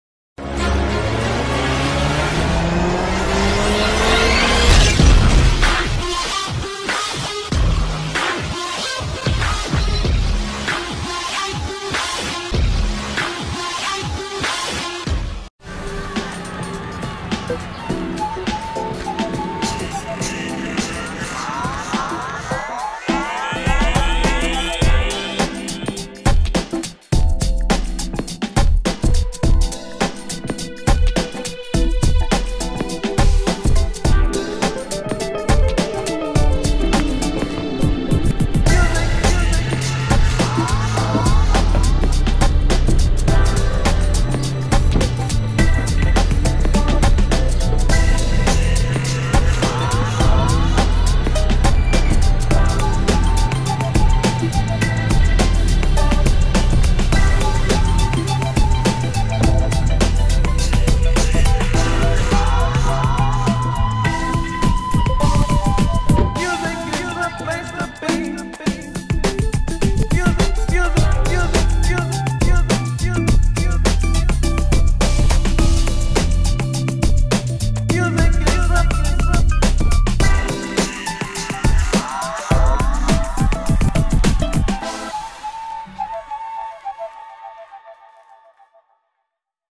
MG5 | Is it really that low? | Ground Clearance | Gutter Test
Is it really that low or just have to do it right next time?
Know your car and learn how it should be maneuvered.